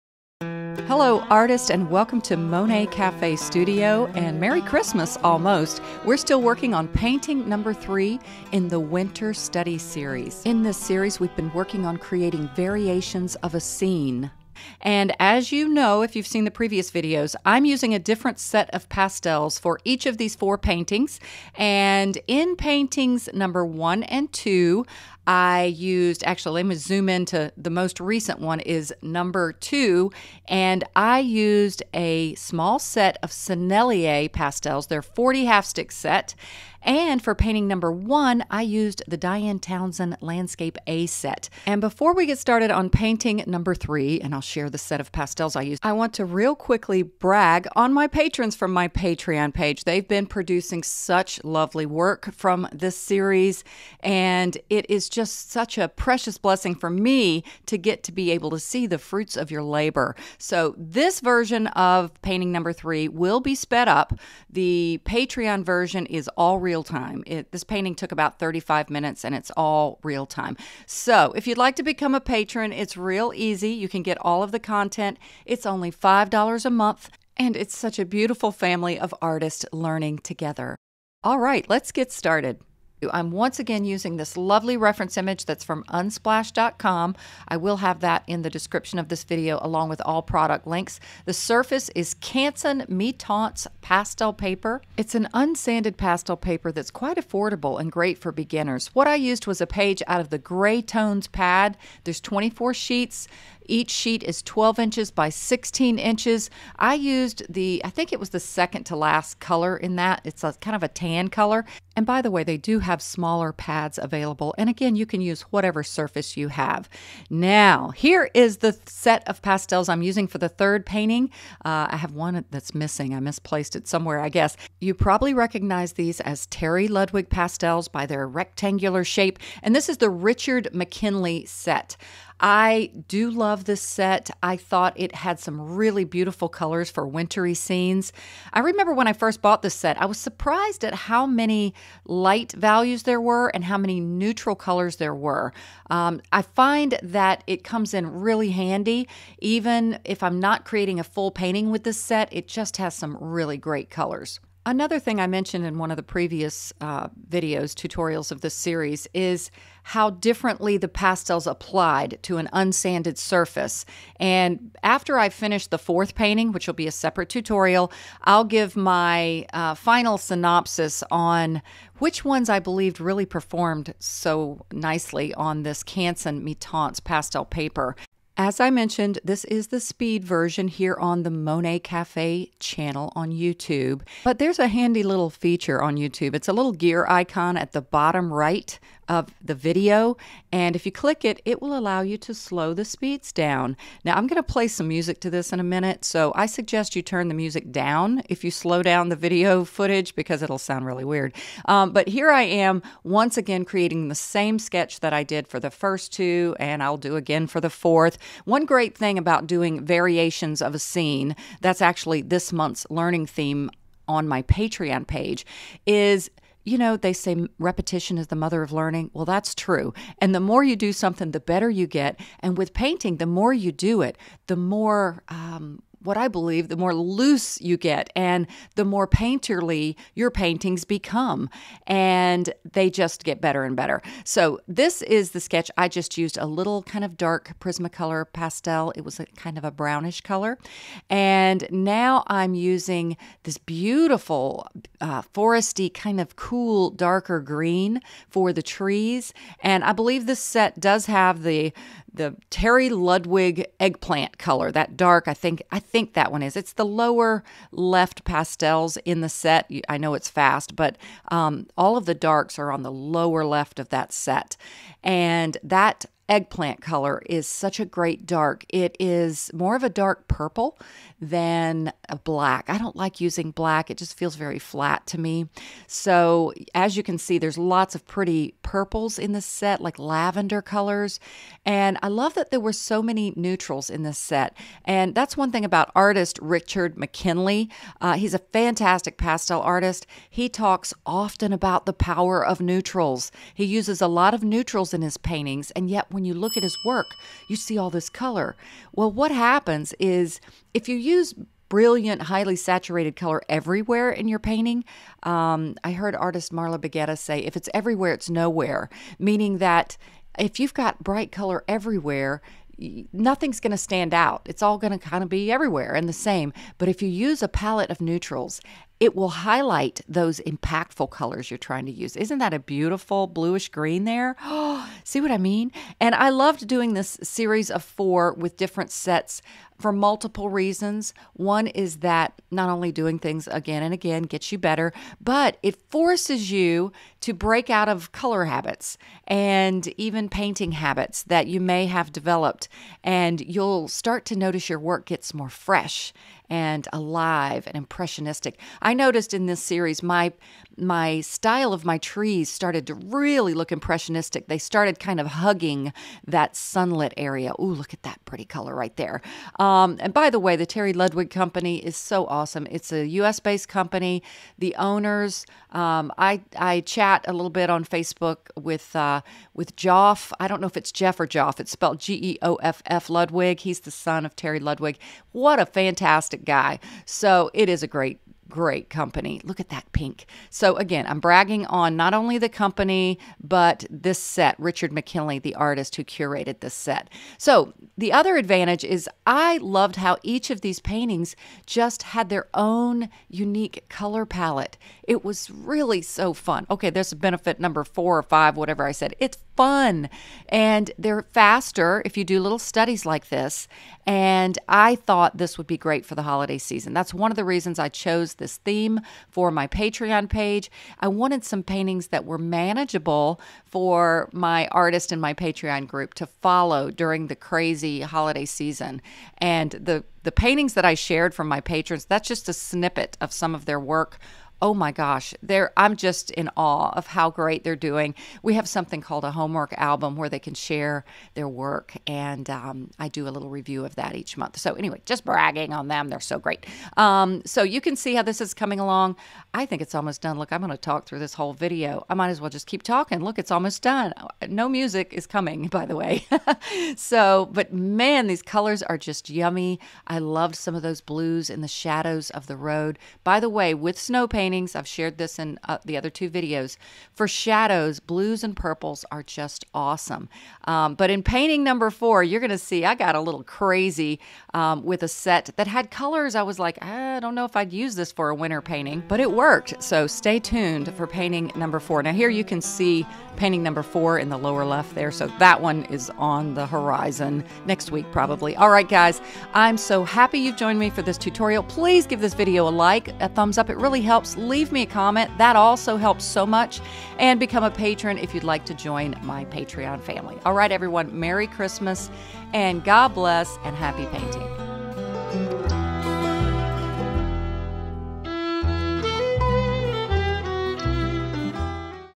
1 Set Of Pastels Is All You Need To Paint A Winter Road - Beginner Friendly Lesson
Hello Artists! I"m excited to bring you painting #3 in my Winter Road painting series. In this series I will be using only 1 set of pastels to create each painting.

❄️ Why focus on one set of pastels? Because great art doesn't always come from an extensive palette. By using a limited palette we can learn to embrace limitations, explore color harmony, and create stunning effects with a condensed set of pastels.

🌨  In this third lesson, I'll be showcasing the Richard McKinley set of Terry Ludwig pastels.    The surface of choice for this artistic journey is once again the practical Canson MiTientes unsanded paper. Affordable and beginner-friendly, this paper is perfect for those starting out with soft pastels.

So, gather your pastels, and let the creative journey begin. Your winter wonderland masterpiece awaits!

✨ Products in This Lesson:

Canson MiTientes Unsanded Pastel Paper / Gray Tone Pad / 12" x 16"

(I earn money from qualifying purchases at no cost to you)